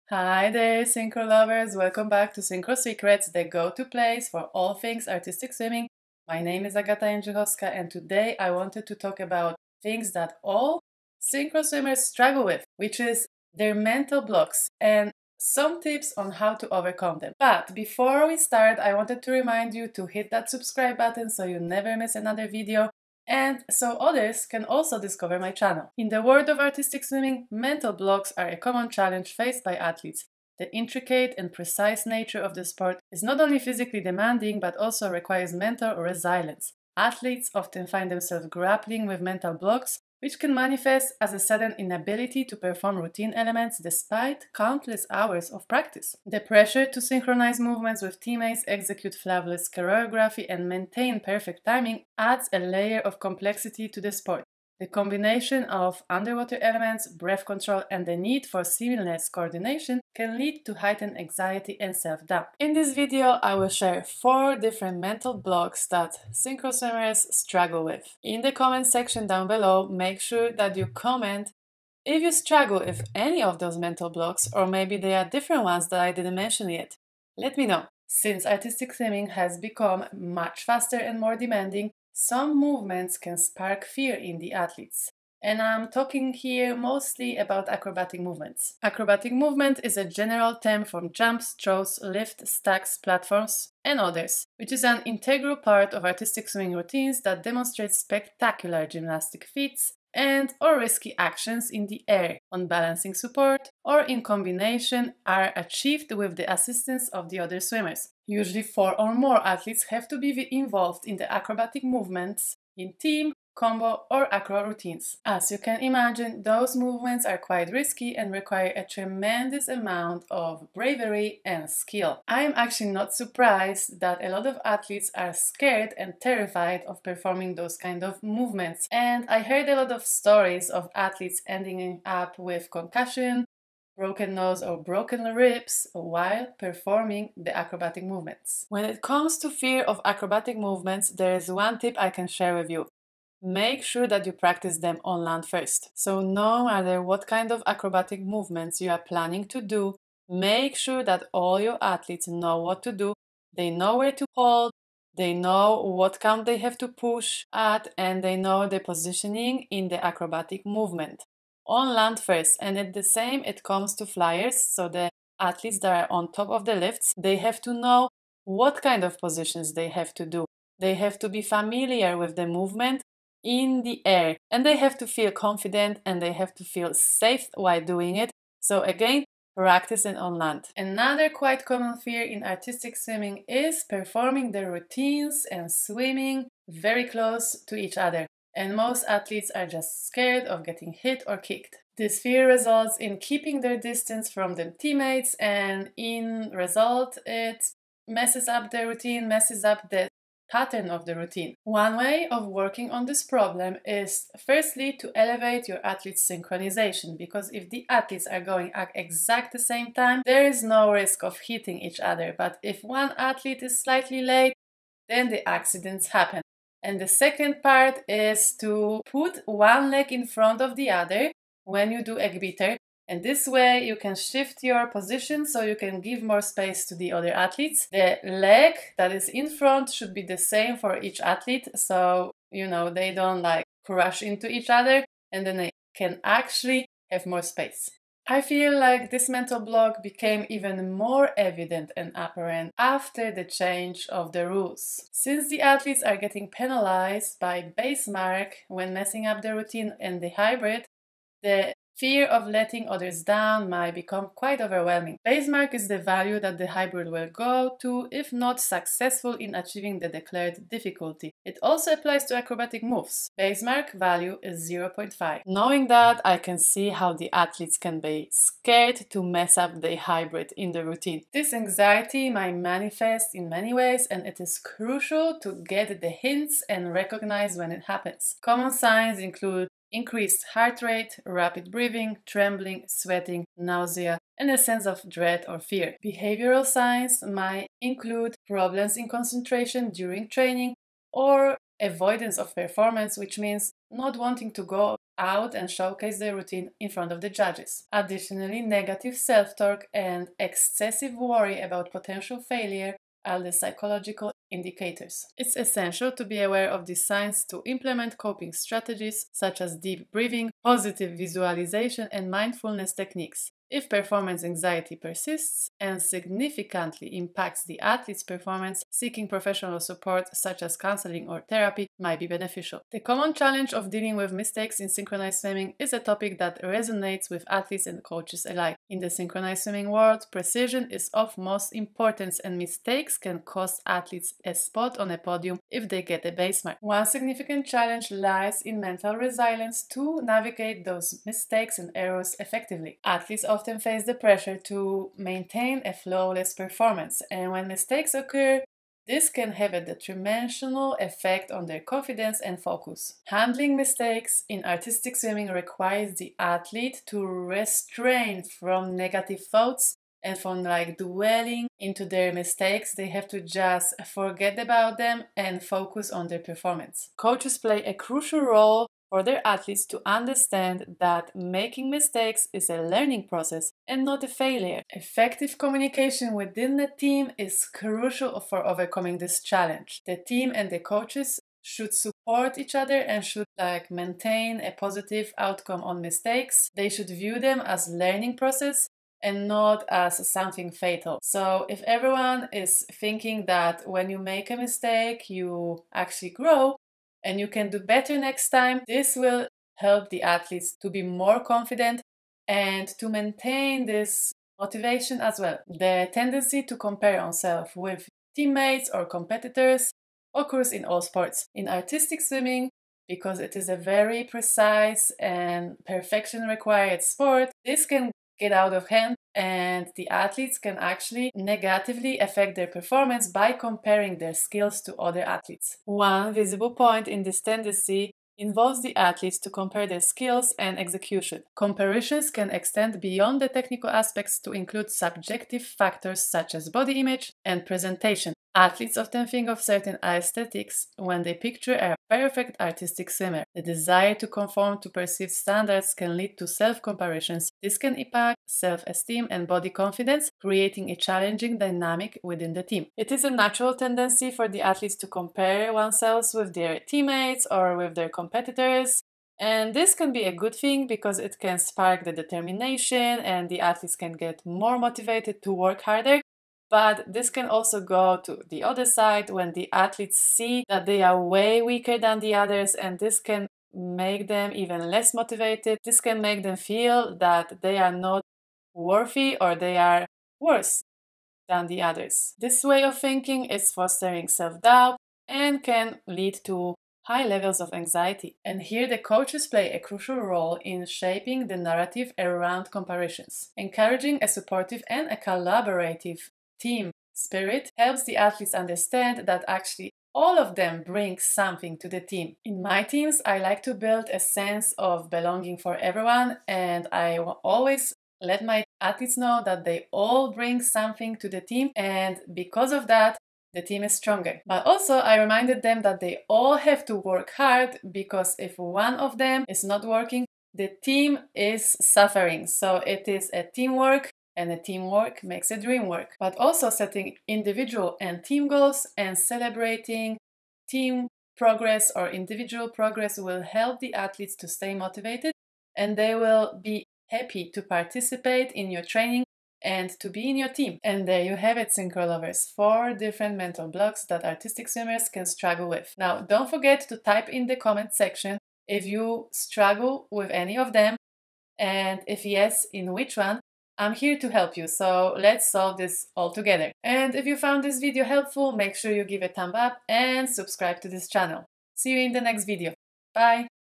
4 Mental Blocks that Artistic Swimmers Struggle With
🧜‍♀️ In this video I am sharing four common mental blocks among artistic swimming athletes.  ✨

Wanna work with me?